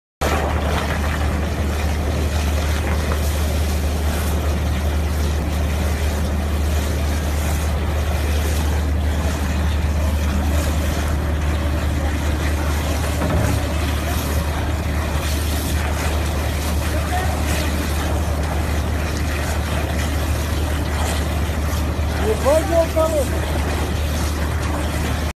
ONSHORE DRILLING | How does it starts | PETROLEUM ENGINEERING
Short video on How the drilling job starts